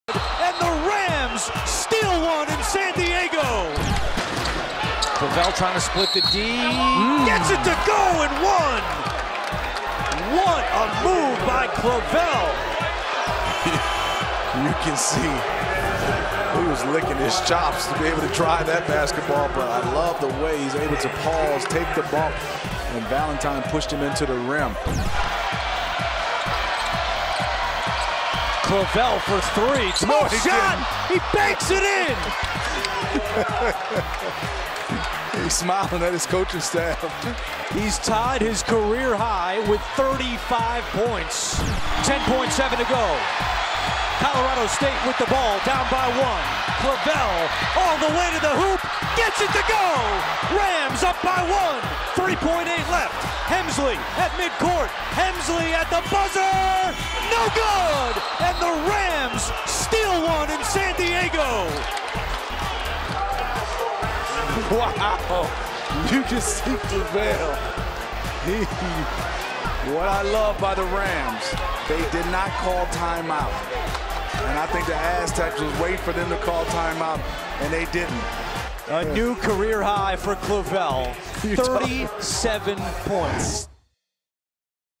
Gian Clavell's Late-Game Heroics For Colorado State | CampusInsiders
Colorado State's  Gian Clavell had a huge night against San Diego State. Trailing by 10 at the half, the senior guard hit three key shots late in the game, including the game-winner with less than 4 seconds left in the game. Clavell finished with a career-high 37 points in the 78-77 victory over the Aztecs.